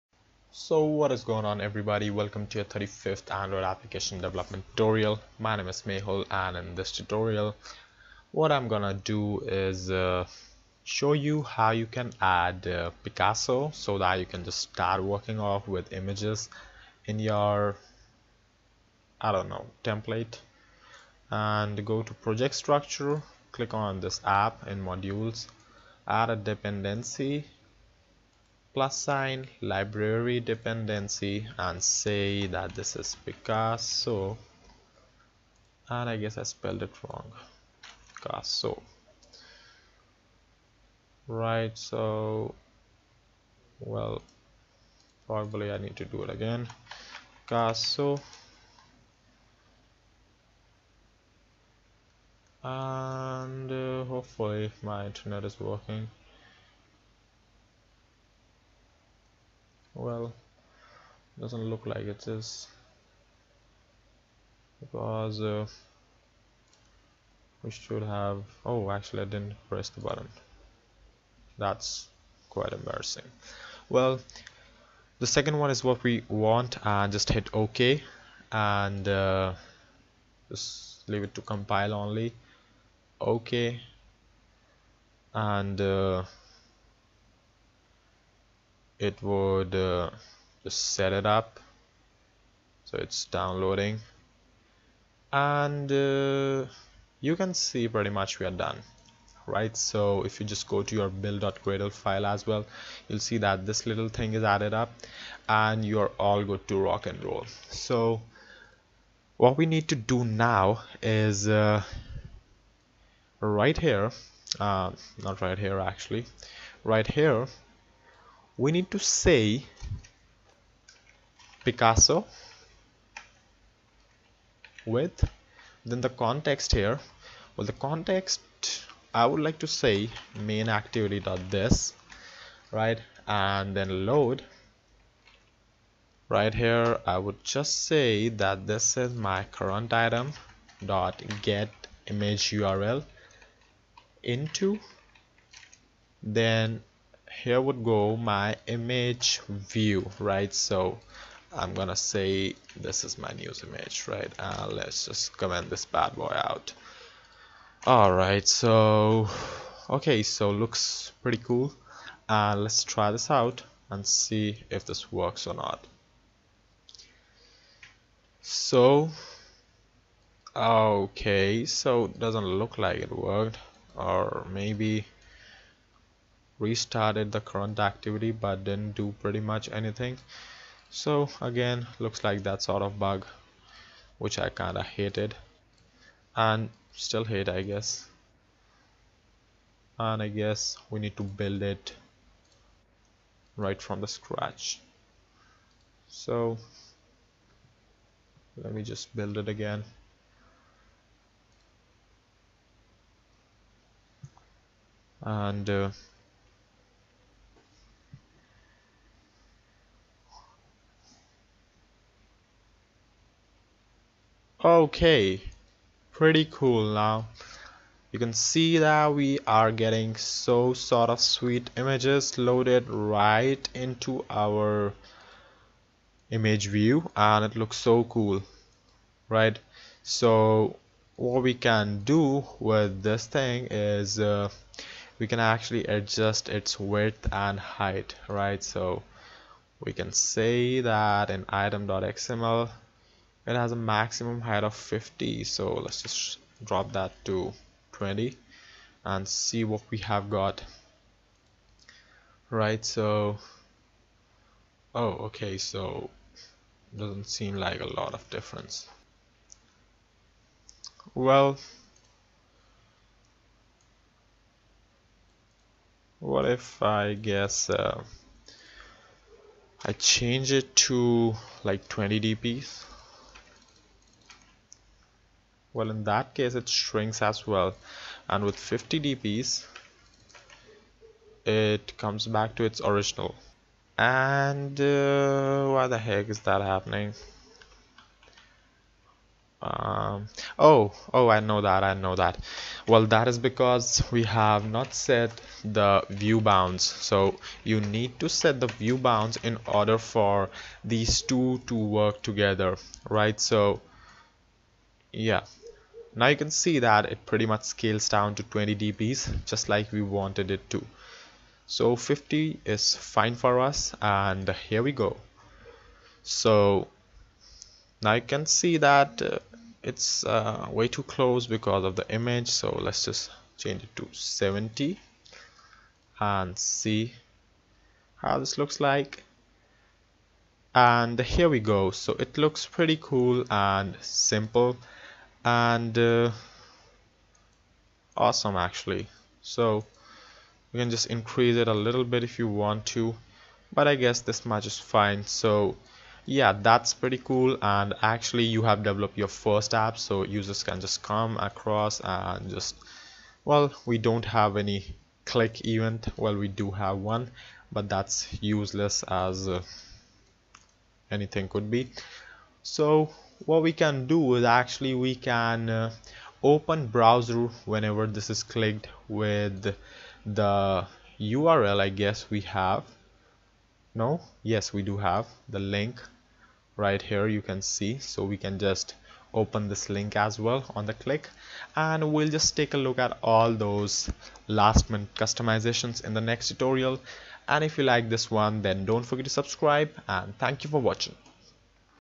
Android App Development Tutorial 35: Adding Picasso To Project ~ Part 6
Learn how to add and work with Picasso image library in Android and set it up for your android app project.